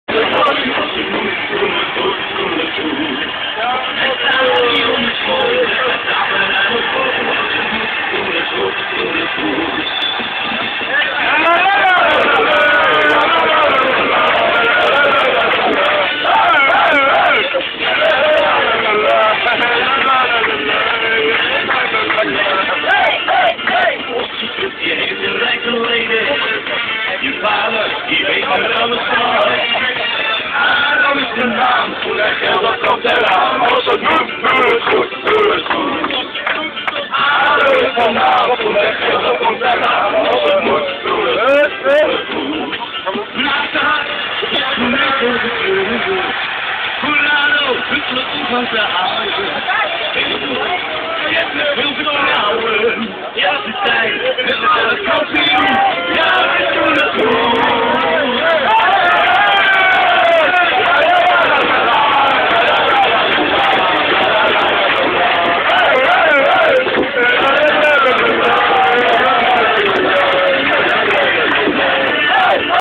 busje 2 naar NAC (FCDH on toer)
ado den haag moet voor een uit wedstrijd naar NAC breda, en dit was de sfeer in de bus voor de wedstrijd.